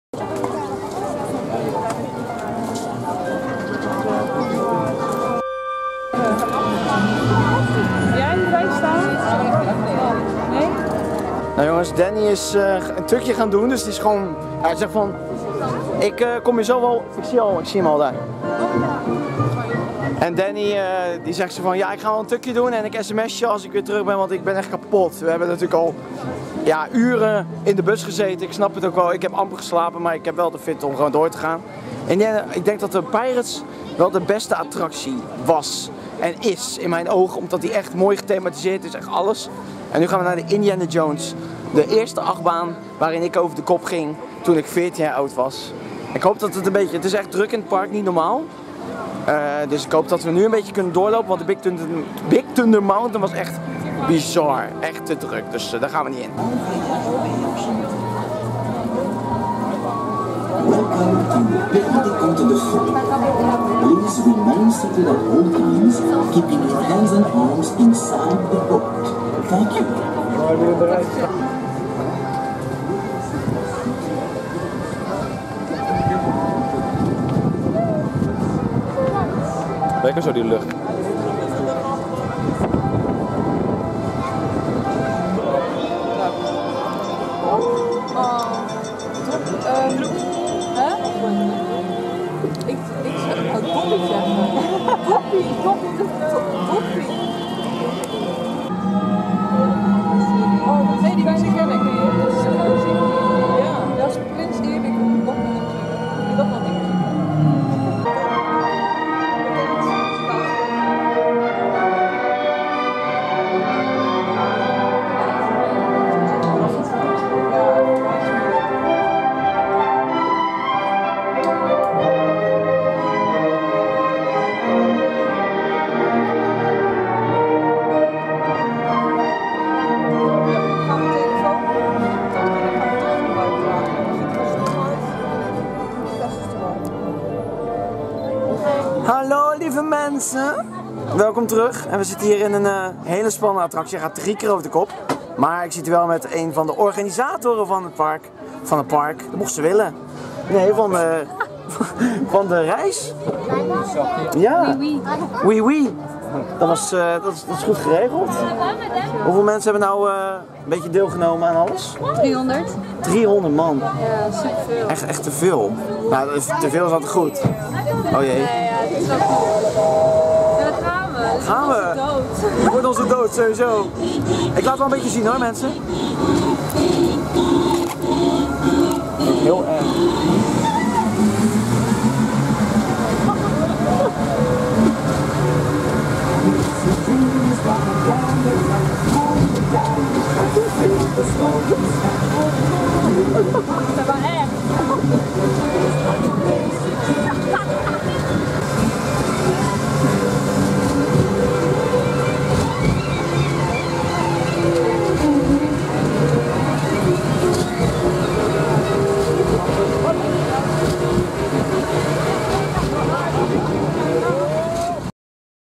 DISNEYLAND TRIP #3
➤ TEAMSPEAK - vincent.disneycraft.net
➤ SERVER IP - vincent.disneycraft.net

➤ [BESCHRIJVING] - 21 Juni 2014 ging ik via school naar Disneyland Parijs. Natuurlijk heb ik mijn camera meegenomen en gaan jullie een aantal leuke filmpjes zien van Disney op mijn kanaal.